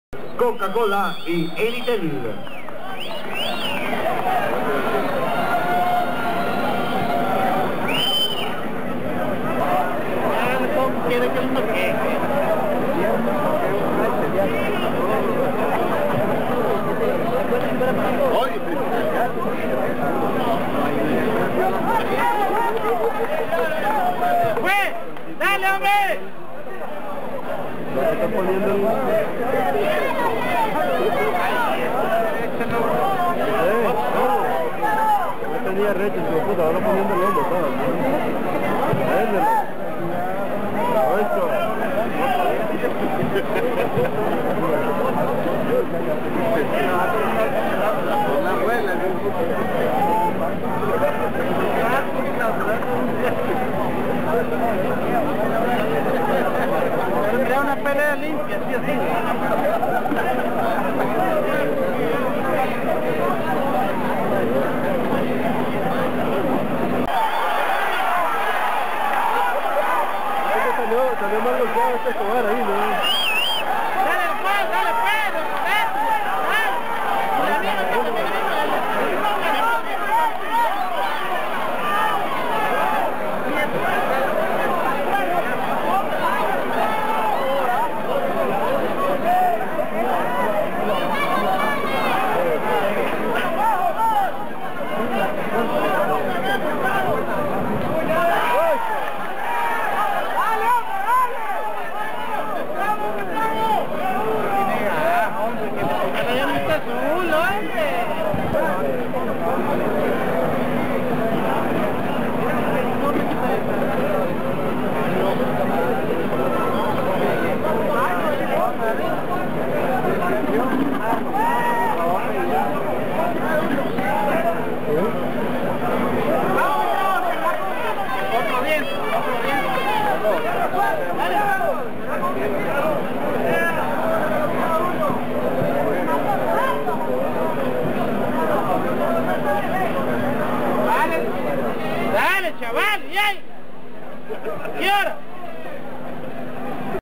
Combate Yader Escobar- Julio Buitrago
Yader Escobar se impuso a Julio "El Halconcito" Buitrago por decisión unánime en la cartelera organizada por Casino Pharaohs y Prodesa en Managua.